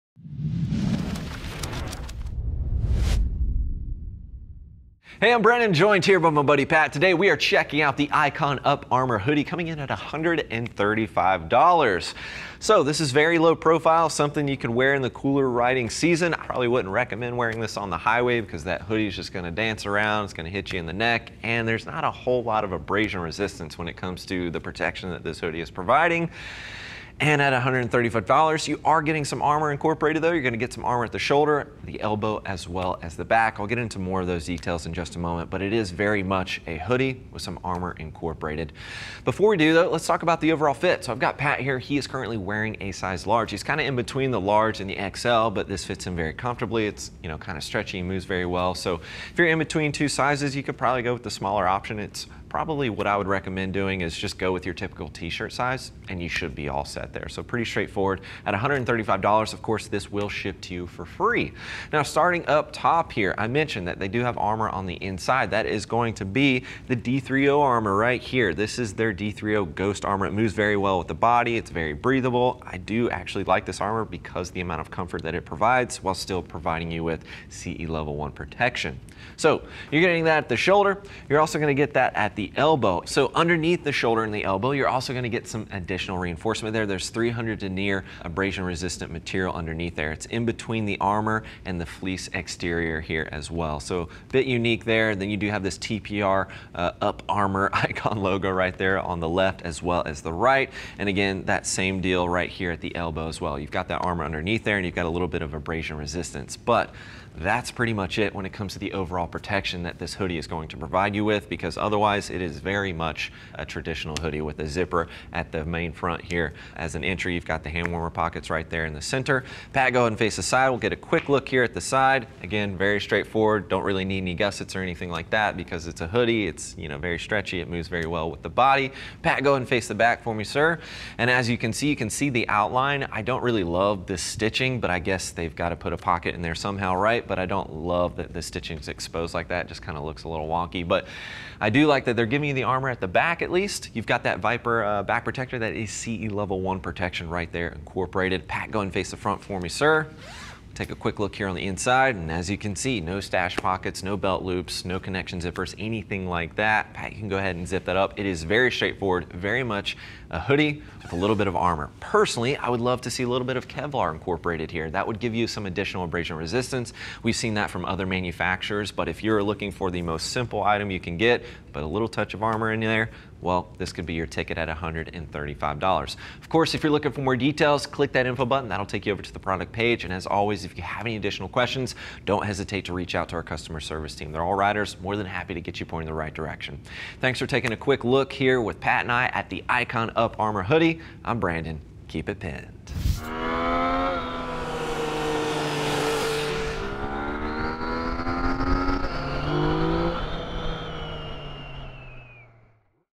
Icon Uparmor Hoody Review
Looks like a hoody, protects like real moto gear. The Icon Uparmor hides D30 shoulder/elbow armor and a Viper back protector under the familiar silhouette of your favorite sweatshirt. Comfortable10 oz cotton has internal 300D polyester reinforcements in elbows and shoulders in case you hit the deck. Subtle reflective accents add some visibility when riding at night.